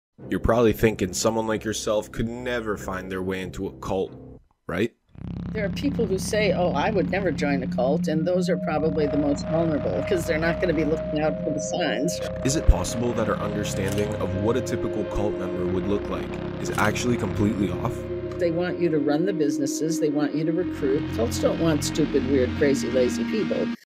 The truth about people in cults
A common misunderstanding when it comes to people caught in cults is that they are lazy, stupid and misguided people. The truth of the average cult member is actually far from this and Dr. Lalich explains, they search for actually the quiet opposite as the goal isn't just recruitment but for a role to be filled and carried out.